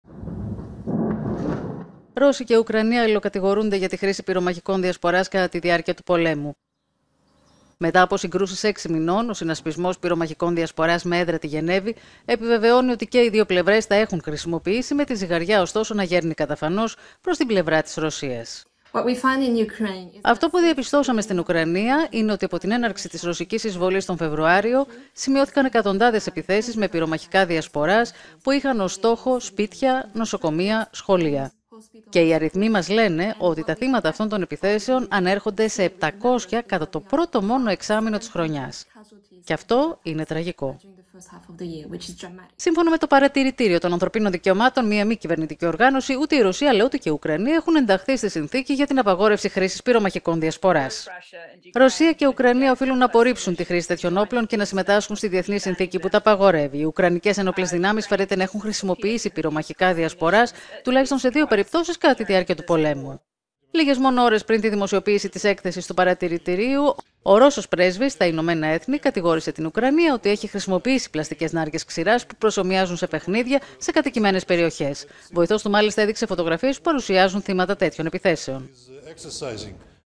Ρωσία και Ουκρανία κάνουν χρήση πυρομαχικών διασποράς, καταγγέλλει ευρωπαϊκός οργανισμός…
Καμία από τις δύο χώρες δε συμμετέχει στη σύμβαση απαγόρευσης τέτοιων όπλων